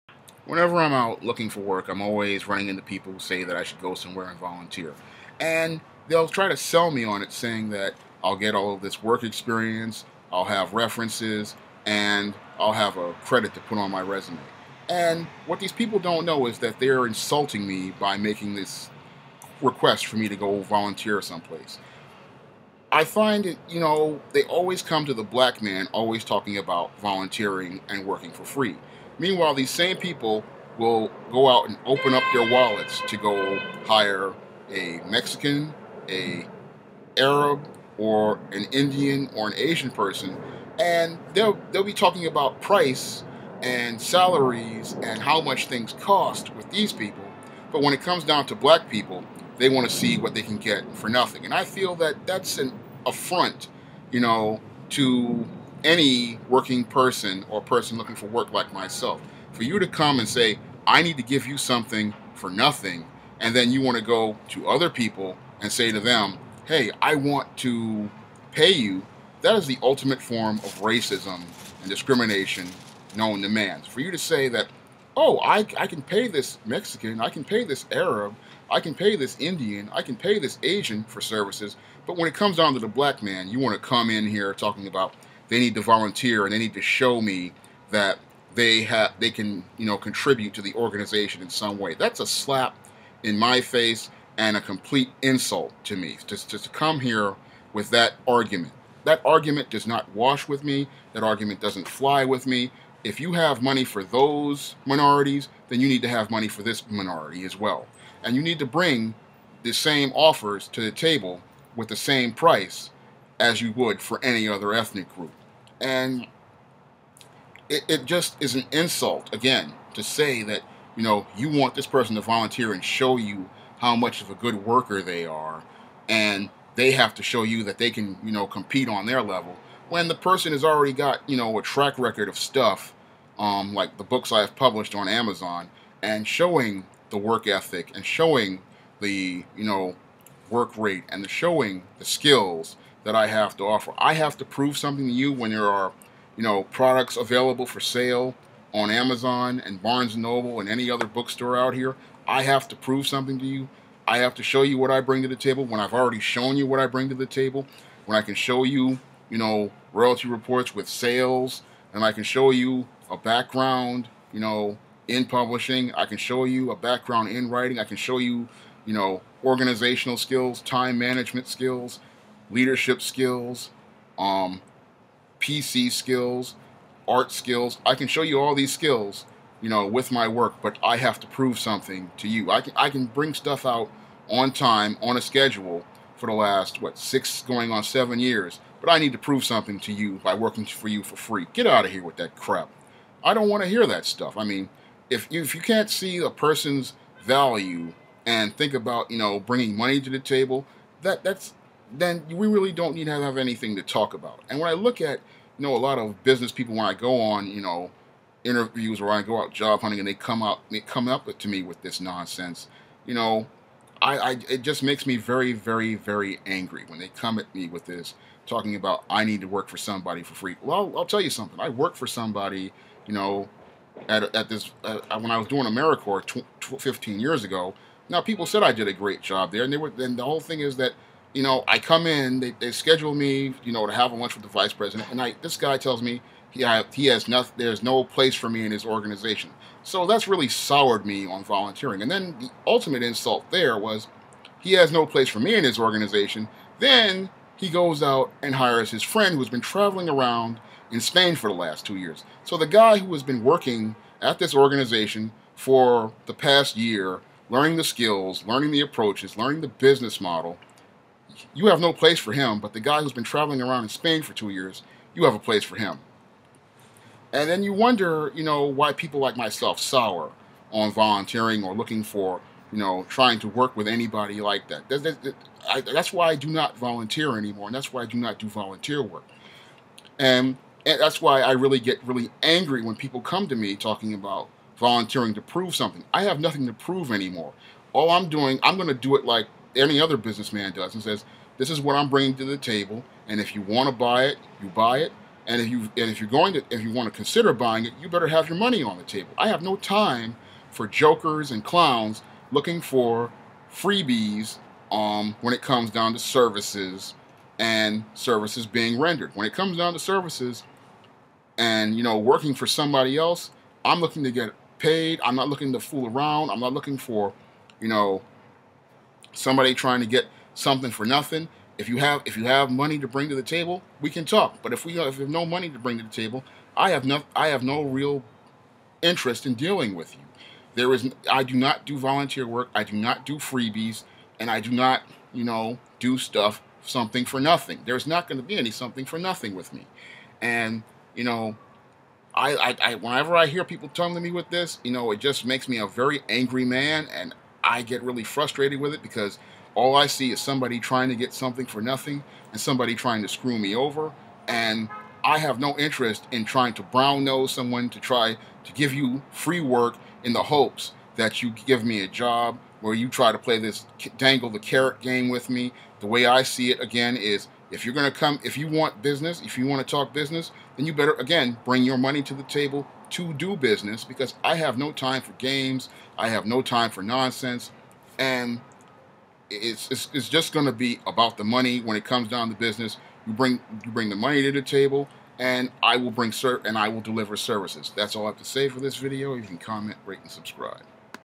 Devaluing a Black Man's Work
Why is it when the Black man comes looking for work no one wants to talk money, but instead want to talk shit.